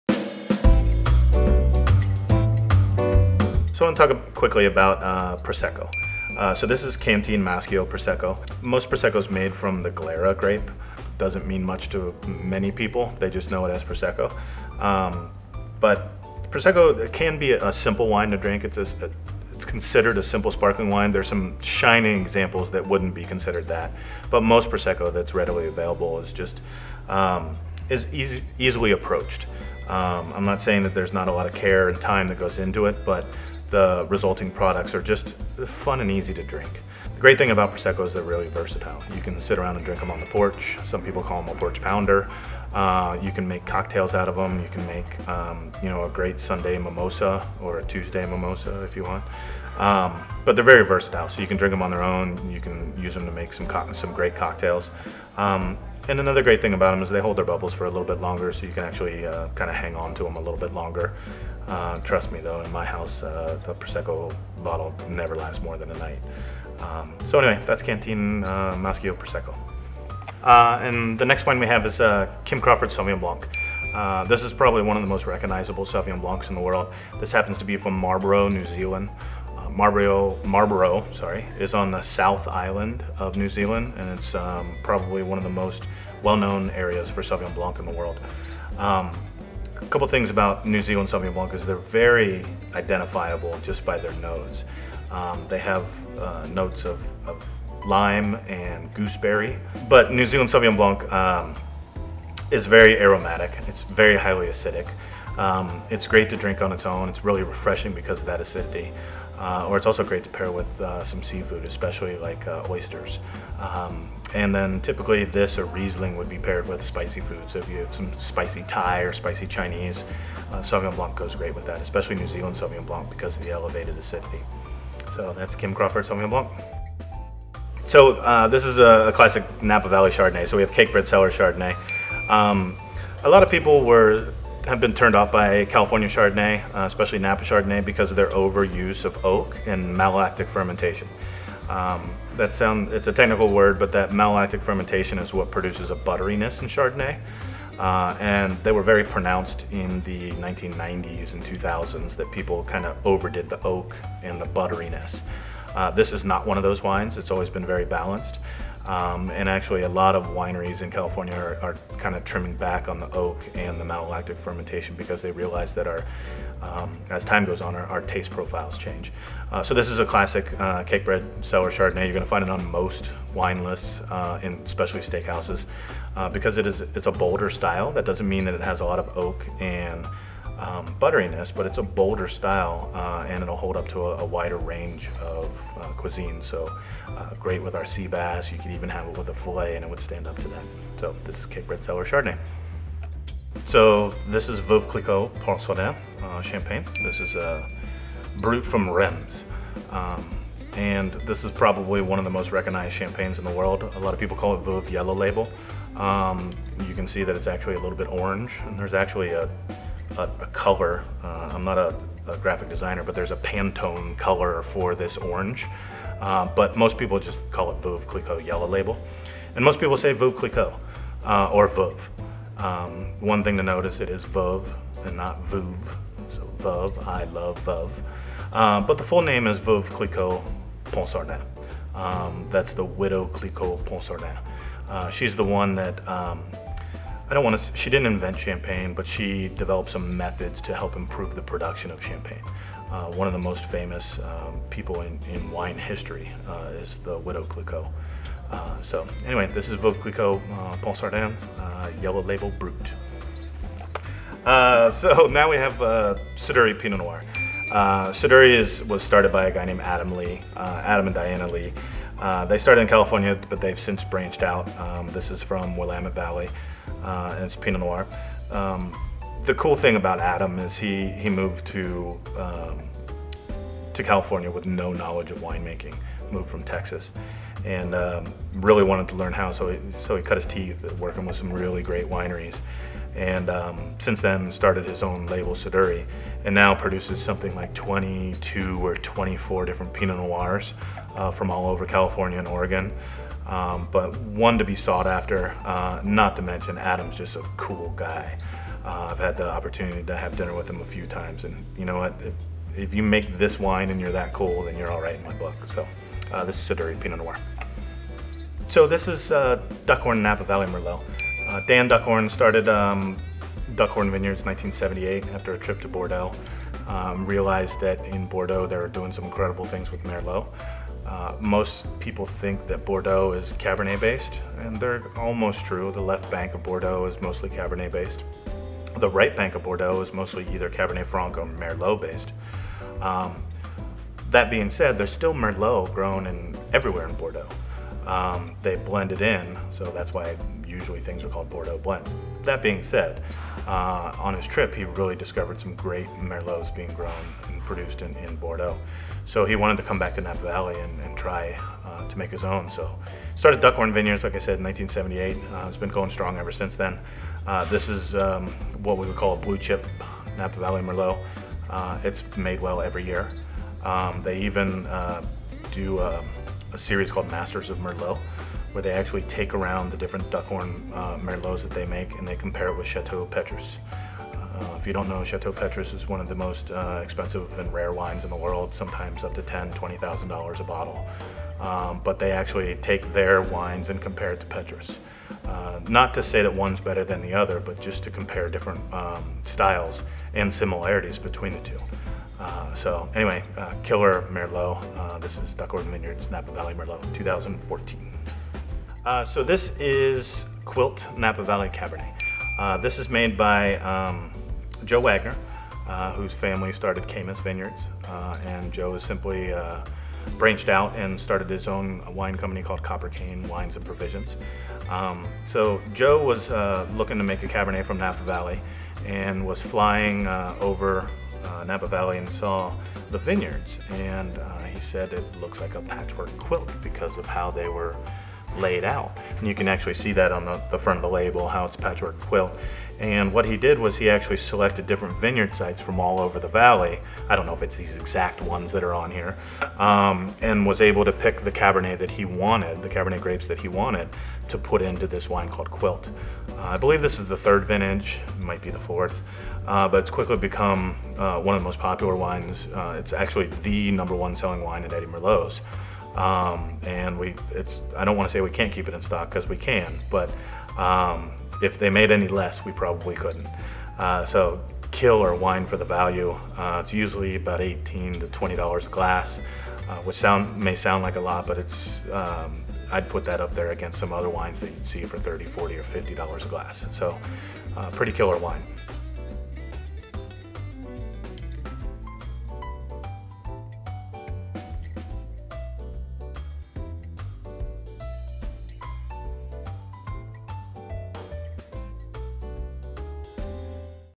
Bretton Talks Wine - 7 Varietals & Producers
Eddie Merlot's Sommelier, Bretton Lammi, shares some information on Prosecco, Sauvignon Blanc, Chardonnay, Champagne, Pinot Noir, Merlot, Cabernet.